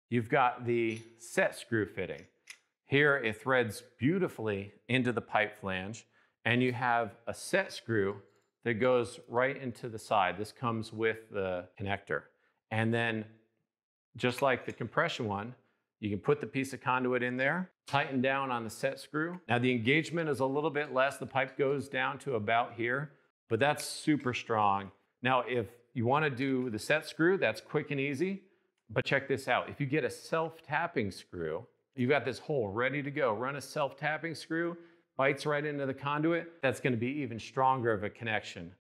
Tips for a stronger Flange in your EMT Conduit builds.
Dave of MakerPipe explains how to use a set screw to improve the durability of your flange in a few different ways.  What will you build?